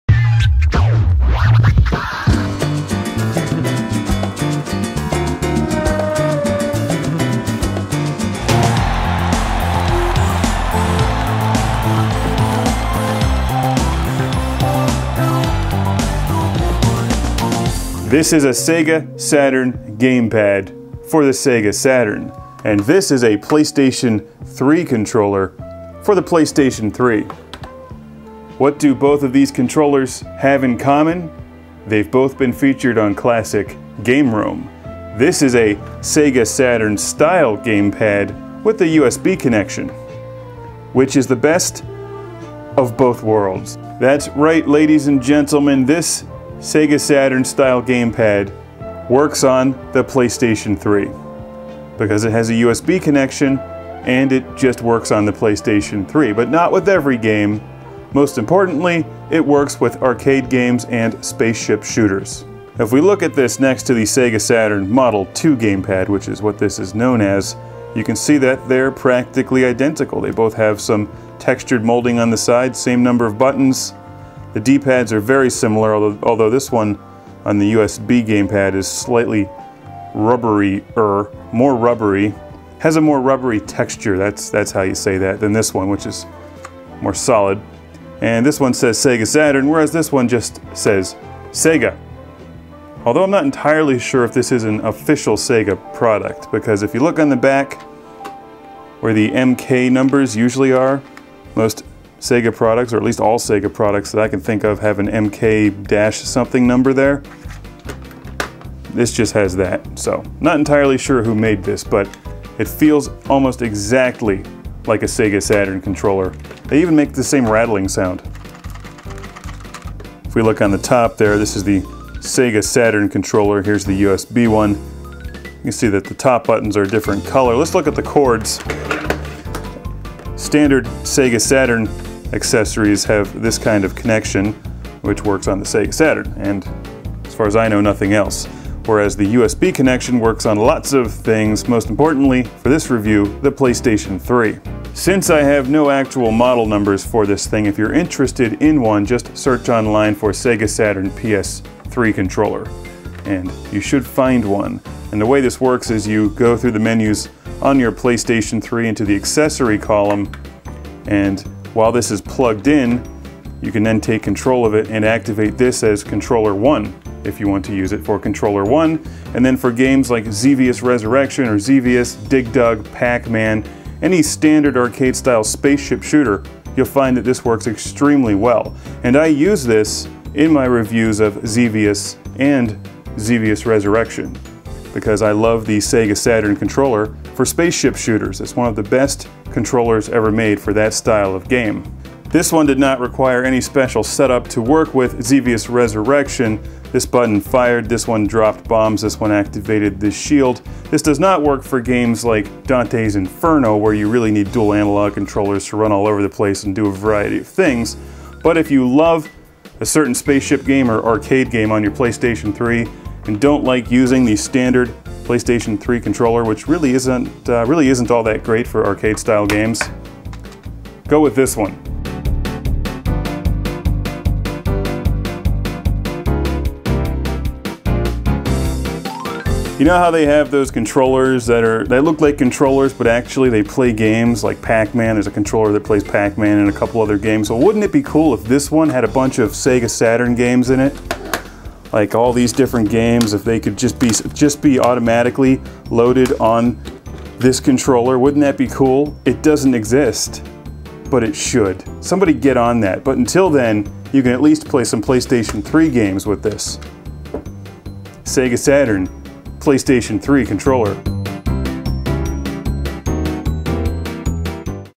Classic Game Room HD - SEGA SATURN PS3 CONTROLLER review
Classic Game Room HD reviews the SEGA SATURN PS3 Controller for Playstation 3 PS3 game console (also works for PC games and MAME arcade video games). This Sega Saturn style controller has a USB connection which allows it to work with the PS3. The Sega Saturn PS3 controller is ideal for space ship shooters (SHMUPS) like Xevious Resurrection and others. Get that real Saturn style gamepad with a slick design and easy to use d-pad. PS3 joystick fighting game pad gamepad modded controller.